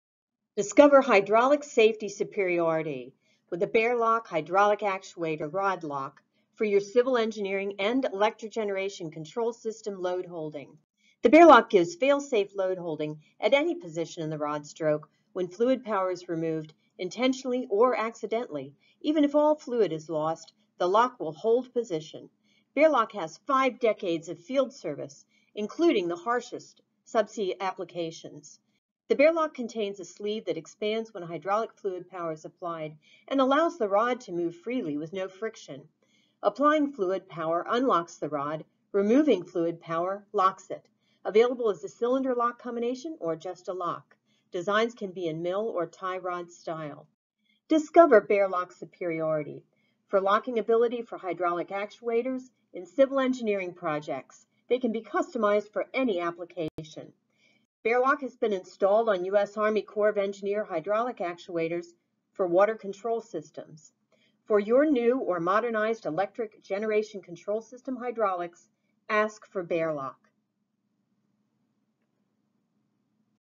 Hydraulic Safety For Civil Engineering & Hydro-Control - Bear-Loc
Discover hydraulic safety superiority with the Bear-Loc hydraulic actuator rod lock for your civil engineering and electric generation control system load holding. The Bear-Loc gives fail-safe load holding at any position in the rod stroke when fluid power is removed intentionally or accidentally even if all fluid is lost, the lock will hold the position. Bear-Loc has five decades of field service including the harshest subsea applications. The Bear-Loc contains a sleeve that expands when hydraulic fluid power is applied and allows the rod to move freely with no friction. Applying fluid power unlocks the rod, and removing fluid power locks the rod. It's available in the cylinder lock combination or just a lock design and it can be in mill or tie rod style.

Discover bear lock superiority for the locking ability of hydraulic actuators in civil engineering projects. They can be customized for any application. Bear-Loc has been installed on U.S.A. army corps of Engineers hydraulic actuators, for water control systems. For your new or modernized electric generation control system hydraulics, ask for Bear-Loc.